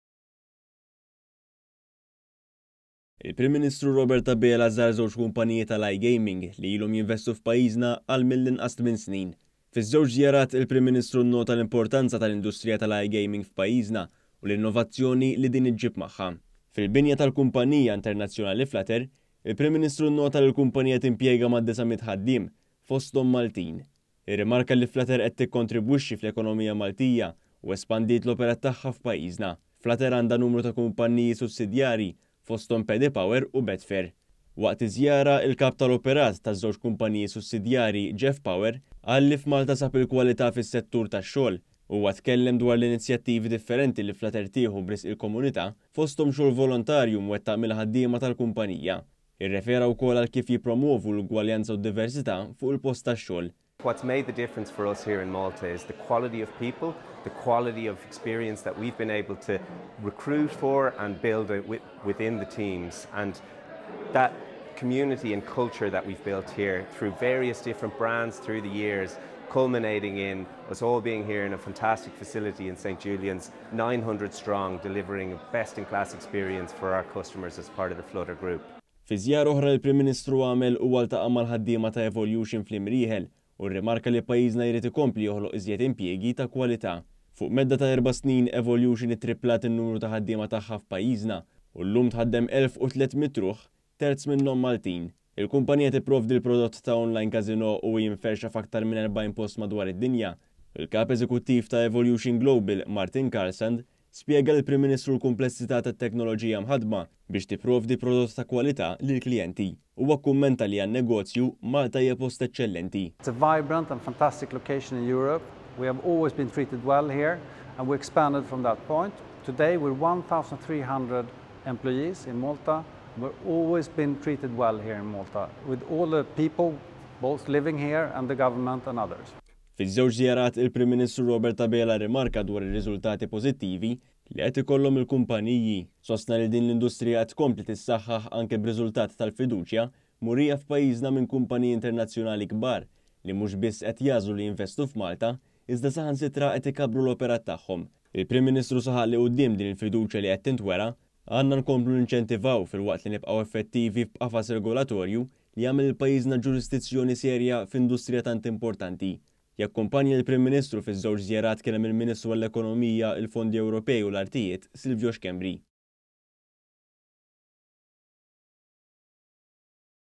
ŻEWĠ KUMPANIJI TAL-IGAMING IŻIDU L-IMPJIEGI F'PAJJIŻNA
ONE.COM.MT • Il-Prim Ministru, Robert Abela, żar żewġ kumpaniji tal-iGaming li ilhom jinvestu f’pajjiżna għal mill-inqas tmien snin.

Fiż-żewġ żjarat, il-Prim Ministru tkellem dwar l-importanza ta’ dan il-qasam kif ukoll dwar l-impenn tal-Gvern li jibqa’ joħloq impjiegi ta’ kwalità.

KITBA: SANDRO MANGION
FILMATI: OPM

Waqt żjara f’żewġ kumpaniji li joperaw fil-qasam tal-iGaming, il-Prim Ministru Robert Abela nnota l-importanza tal-industrija tal-iGaming f’pajjiżna u l-innovazzjoni li din iġġib magħha.

Fil-binja tal-kumpanija internazzjonali Flutter, fi Spinola Park, il-Prim Ministru nnota li l-kumpanija timpjega mad-900 ħaddiem, fosthom Maltin. Irrimarka li Flutter qed tikkontribwixxi fl-ekonomija Maltija u espandiet l-operat tagħha f’pajjiżna. Flutter għandha numru ta’ kumpaniji sussidjarji fosthom PaddyPower u Betfair.

Waqt iż-żjara, il-Kap tal-Operat ta’ PaddyPower u Betfair, Jeff Power, qal li f’Malta sab il-kwalità fis-settur tax-xogħol. Huwa tkellem dwar inizjattivi differenti li Flutter tieħu b’risq il-komunità, fosthom xogħol volontarju mwettaq mill-ħaddiema tal-kumpanija.

Irrefera wkoll għal kif jippromovu l-ugwaljanza u d-diversità fuq il-post tax-xogħol.

JEFF POWER
KAP TAL-OPERAT TA’ PADDYPOWER U BETFAIR
“What has made the difference for us here in Malta is the quality of people, the quality of experience that we have been able to recruit for an build within the teams and that community and culture that we have built here thrugh various different brands through the years culminating in us all being here in this fantastic facility here in St Julians. 900 people strong delivering best in class experience for our customers as part of the flutter group.”

Fi żjara oħra li l-Prim Ministru għamel, huwa ltaqa’ mal-ħaddiema ta’ Evolution, fl-Imrieħel, u rrimarka li pajjiżna jrid ikompli joħloq iżjed impjiegi ta’ kwalità. Fuq medda ta’ erba’ snin, Evolution ittripplat in-numru ta’ ħaddiema tagħha f’pajjiżna u llum tħaddem elf u tliet mitt ruħ, terz minnhom Maltin.

Il-kumpanija tipprovdi l-prodott ta’ online casino u hi mferrxa f’aktar minn erbgħin post madwar id-dinja. Il-Kap Eżekuttiv ta’ Evolution Global, Martin Carlesund, spjega lill-Prim Ministru l-kumplessità tat-teknoloġija mħaddma biex tipprovdi prodott ta’ kwalità lill-klijenti. Huwa kkummenta li, għan-negozju, Malta hija post eċċellenti.

MARTIN CARLESUND
KAP EŻEKUTTIV TA’ EVOLUTION GLOBAL
“We have always been treated well here and we expanded from that point. Today we are 1,300 employees in Malta. We have always been treated well here in Malta with all the people whilst living here, and the Government and others.”

Fiż-żewġ żjarat, il-Prim Ministru Robert Abela rrimarka dwar ir-riżultati pożittivi li qed ikollhom il-kumpaniji. Sostna li din l-industrija qed tkompli tissaħħaħ anki b’riżultat tal-fiduċja murija f’pajjiżna minn kumpaniji internazzjonali kbar li mhux biss qed jagħżlu li jinvestu f’Malta iżda saħansitra qed ikabbru l-operat tagħhom.

Il-Prim Ministru saħaq li, quddiem din il-fiduċja li qed tintwera, għandna nkomplu ninċentivaw filwaqt li nibqgħu effettivi b’qafas regolatorju li jagħmel lill-pajjiżna ġuriżdizzjoni serja f’industrija tant importanti.

Jakkumpanja lill-Prim Ministru fiż-żewġ żjarat kien hemm il-Ministru għall-Ekonomija, il-Fondi Ewropej, u l-Artijiet, Silvio Schembri.